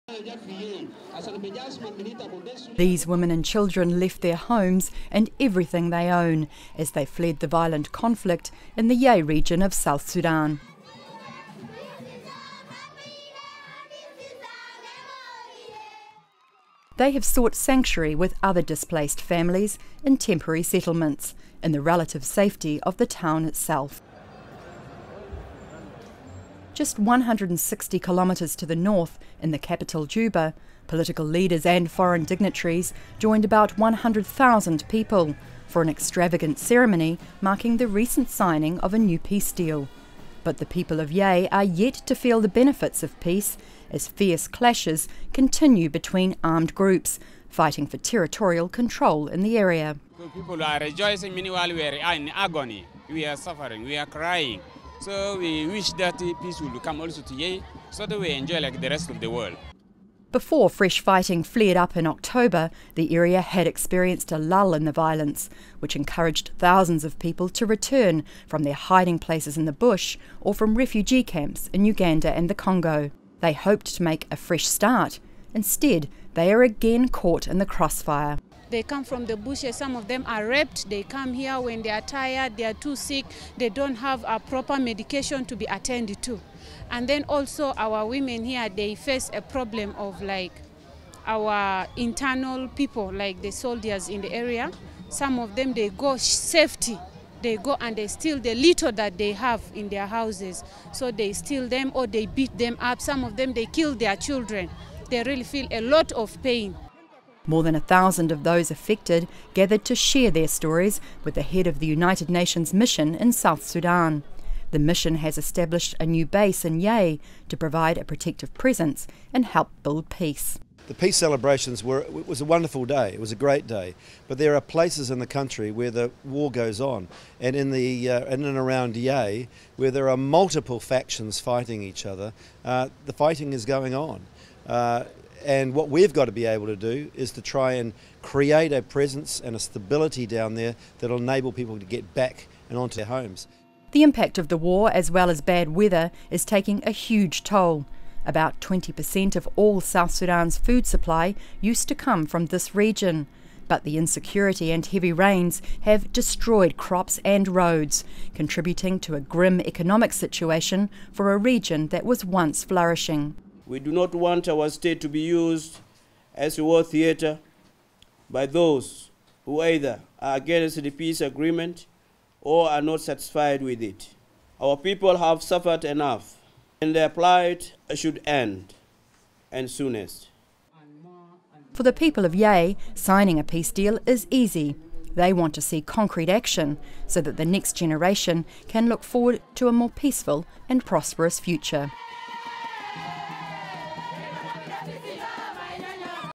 PEOPLE OF YEI YET TO BENEFIT FROM NEW SOUTH SUDAN PEACE DEAL
These women and children left their homes and everything they own as they fled the violent conflict in the Yei region of South Sudan. They have sought sanctuary with other displaced families in temporary settlements in the relative safety of the town itself.

Just 160km to the north, in the capital Juba, political leaders and foreign dignitaries joined about 100,000 people for an extravagant ceremony marking the recent signing of a new peace deal.

But the people of Yei are yet to feel the benefits of peace as fierce clashes continue between armed groups fighting for territorial control in the area.

SOUNDBITE: INTERNALLY DISPLACED PERSON EMMANUEL HAKIM WATA:
“People are rejoicing. Meanwhile, we are in agony. We are suffering. We are crying so we wish the peace would come to Yei so we can enjoy it like the rest of the world.”
Before fresh fighting flared up in October, the area had experienced a lull in the violence which encouraged thousands of people to return from their hiding places in the bush or from refugee camps in Uganda and the Congo. They hoped to make a fresh start. Instead, they are again caught in the crossfire.

SOUNDBITE: SOBE CENTRE SPOKESPERSON KETI VIOLA GEORGE:
“They come from the bush, some of them are raped, they come here when they are tired, too sick, they don’t have proper medication to be attended to and also our women here they face a problem of like our internal people, like the soldiers in the area, some of them go and steal the little they have in the house. So, they steal, or they beat them up, some of them kill their children. They really feel a lot of pain.”

More than a thousand of those affected gathered to share their stories with the Head of the United Nations Mission in South Sudan. The Mission has established a new base in Yei to provide a protective presence and help build peace.

SPECIAL REPRESENTATIVE OF THE SECRETARY-GENERAL, DAVID SHEARER:
“The peace celebration was a wonderful day. It was a great day. But there are places in the country where the war goes on and in and around Yei, where there are multiple factions fighting each other, the fighting is going on and what we’ve got to be able to do is to try and create a presence and a stability down there that will enable people to get back and into their homes.”

The impact of the war as well as bad weather is taking a huge toll. About 20 per cent of all of South Sudan’s food supply used to come from this region. But the insecurity and heavy rains have destroyed crops and roads, contributing to a grim economic situation for a region that was once flourishing.

SOUNDBITE: YEI RIVER DEPUTY GOVERNOR, YOUSTO BEDA DEMA LUKUDU:
“We do not want our state to be used as a war theatre by those who are either against the peace agreement or are not satisfied with it. Our people have suffered enough and their plight should end the soonest.”

For the people of Yei, signing a peace deal is easy. They want to see concrete action so that the next generation can look forward to a more peaceful and prosperous future.